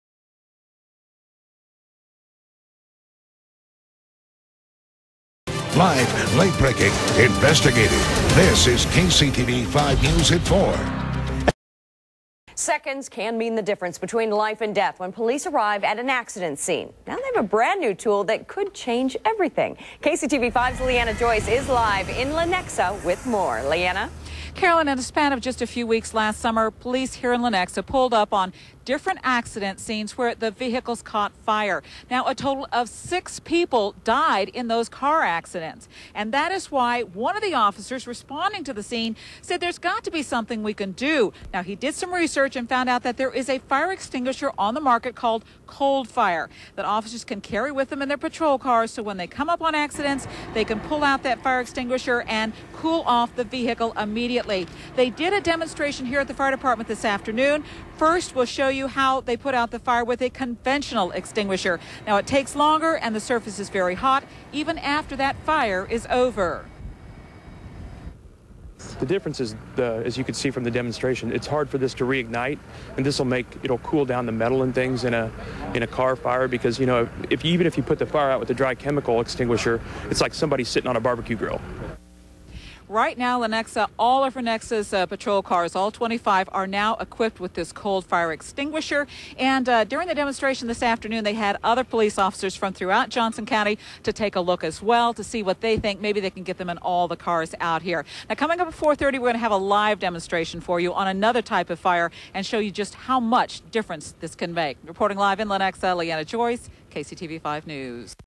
COLD FIRE WEST
Cold Fire West Did A Demo for the Lenexa Police Dept